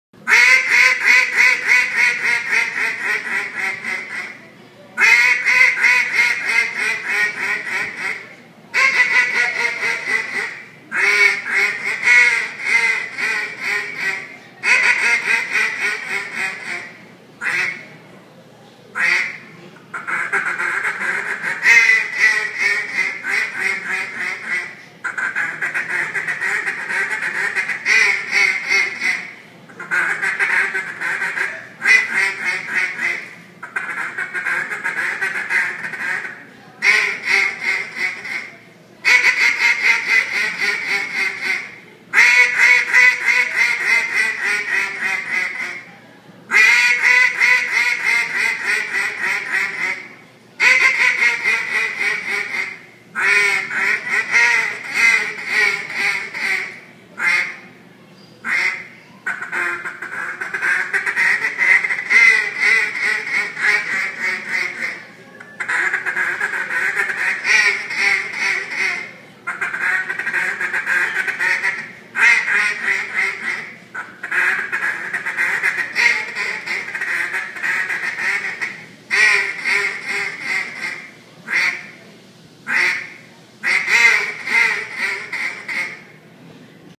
The Duck Woman: Men's Duck Calling World Contest in Katy, TX
The Duck Woman competing in the 2013 World Waterfowl Calling Championship.